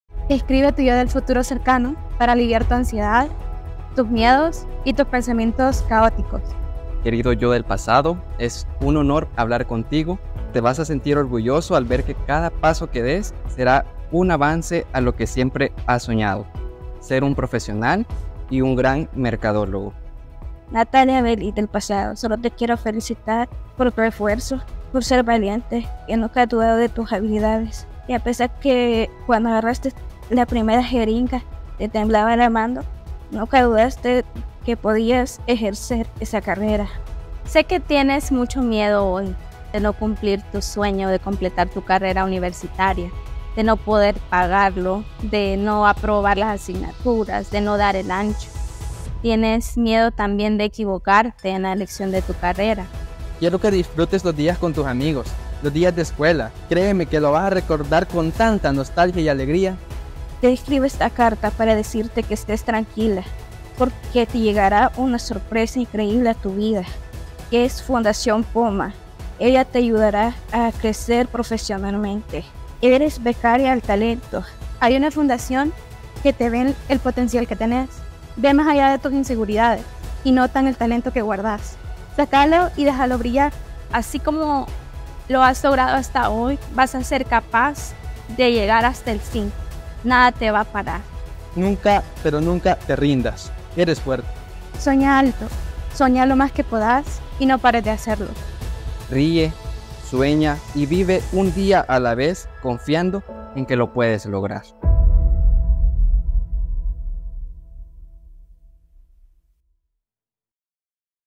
Becas al Talento-Fundación Poma
¡La convocatoria 2025 ya está abierta!

Conoce a nuestros becarios, jóvenes como tú, con fuertes deseos de superación. Ellos dieron el paso para alcanzar sus metas, y los sueños que alguna vez parecieron lejanos se están cumpliendo con el apoyo de Becas al Talento.

En FundaciónPoma, creemos en el potencial que tienes continuar tus estudios superiores.

¡Lo puedes lograr con esfuerzo y perseverancia! ✨

Aplica ya a Becas al Talento, ¡estamos listos para acompañarte en este camino!

📌  El primer paso comienza aquí: becasaltalento.fundacionpoma.org